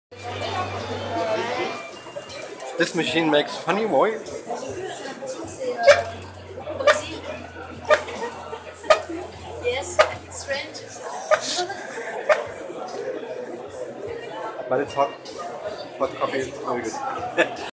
coffee pump with a funny noise
Recorded at the MIDEM 14, Cannes (F), German pavillon